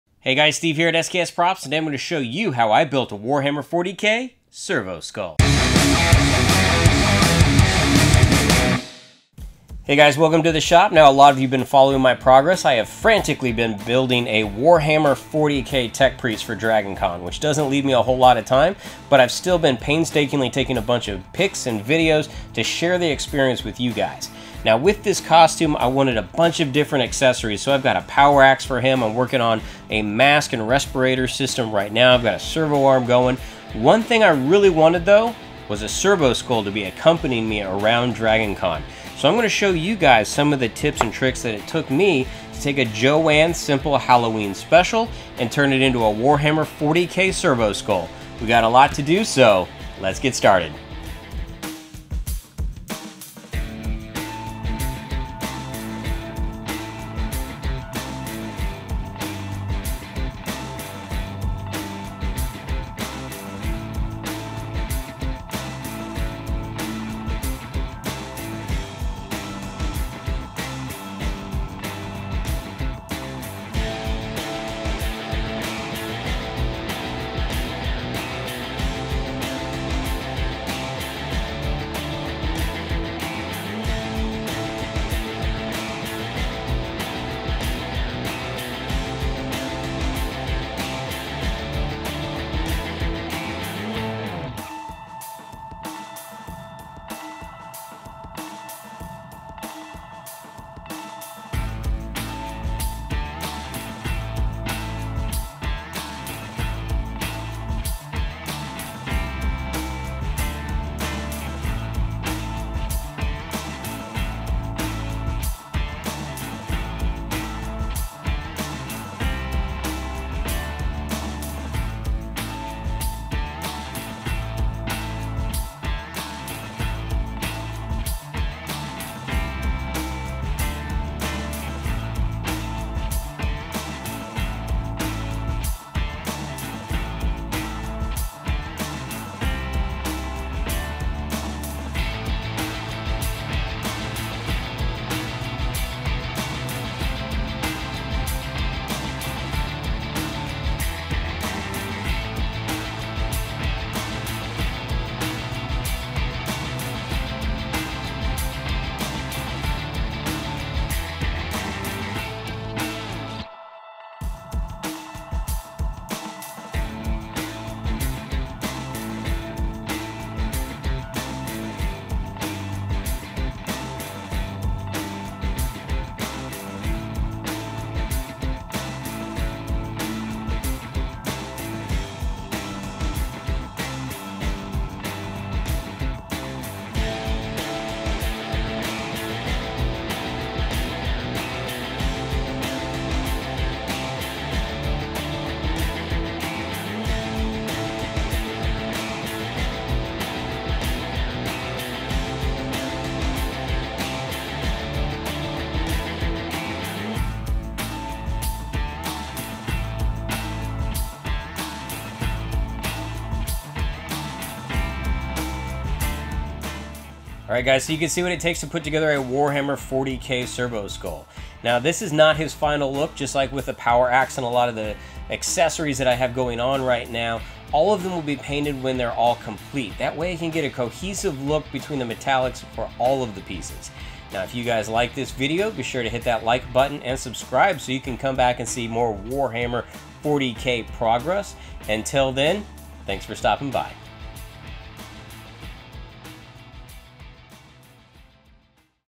How to make a Warhammer 40K Servo Skull Cosplay Prop
Hey everyone welcome to my latest build in my mad dash to get a Warhammer 40K Tech Priest completed for DragonCon.  This time I am focusing on a custom made Servo Skull that will be attached to my pack.  This build wasn't to bad to put together. I just used a bunch of tubing, wires, and a plastic Halloween skull from the craft store. Enjoy!

Be sure to check out the Power Axe videos too!

Links-

Ribbed Pipe

Ribbed Pipe

Disc/Belt Sander

Bob Smith Super Glue 2oz

Bob Smith Glue Accelerator 2oz

Extra Tips for Glue Bottles